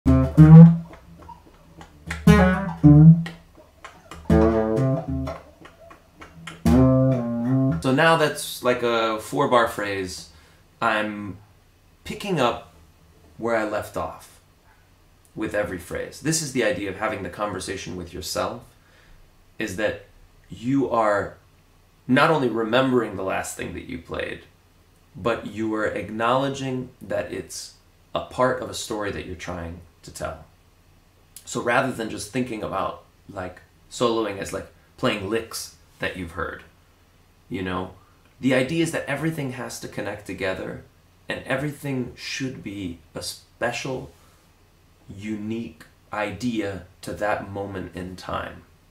Simple. Solo. Strategy (w/ Michael League)
In this clip, the formidable Michael League of Snarky Puppy shares his approach to Telling a Story as a Soloist.

This clip is taken from the Michael League SBL seminar "Telling A Story As A Soloist With Michael League".